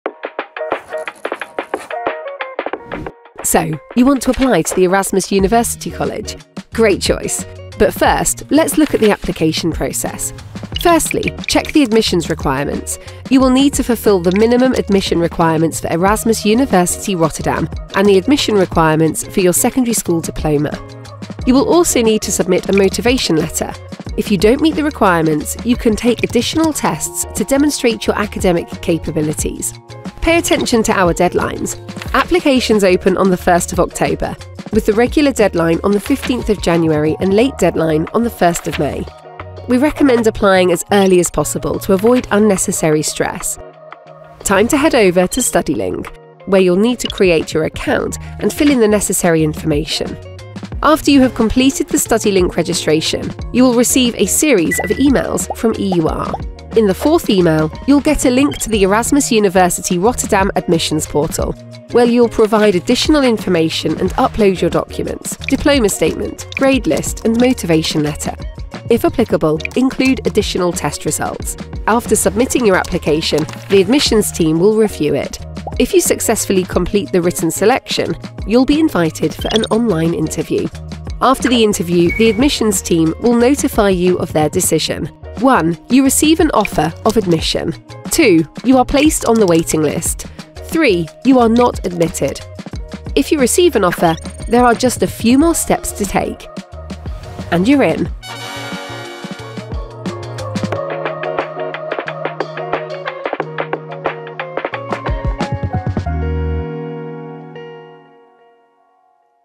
Application Procedure | Erasmus University College
Get an overview of the application procedure to the Bachelor in Liberal Arts & Sciences at Erasmus University College.

EUC accepts 218 first-year students per year. As a challenging bachelor, we carefully evaluate candidates for our program, keeping in mind their grades and letter of motivation. However, we look beyond the numbers and seek individuals who demonstrate passion, curiosity, and a genuine desire to learn. Our selection process aims to understand your unique potential and how you can contribute to our dynamic academic environment.

Here's our application procedure in short:
1.Register with Studielink, the Dutch student database, (available in English).
2.Submit your grade list and motivation letter through our application portal.
3.Applications meeting our minimum entry requirements will be reviewed.
4.Successful candidates will be invited for an online interview.
5.For detailed information about the application procedure and admission requirements,